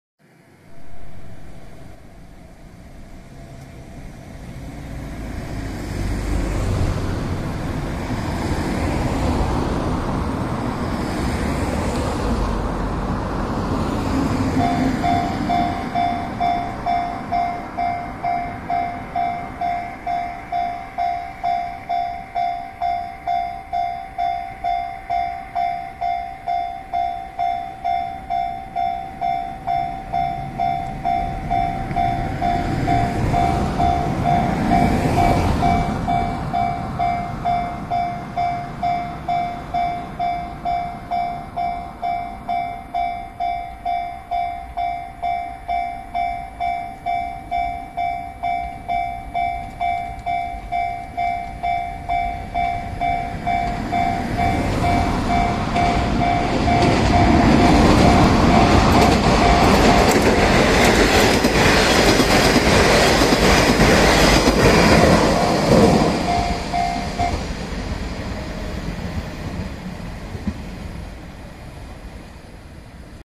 明石南踏切を通過する1200型気動車、その3。
明石南踏切の日常動画、その3です。

こちらは、1200型気動車メインの動画です。

こちらの踏切は、昨日撮影したばかりの動画です。

今回も、反対方向から撮影して見ました。

場所は同じですので、以下の説明は省略します。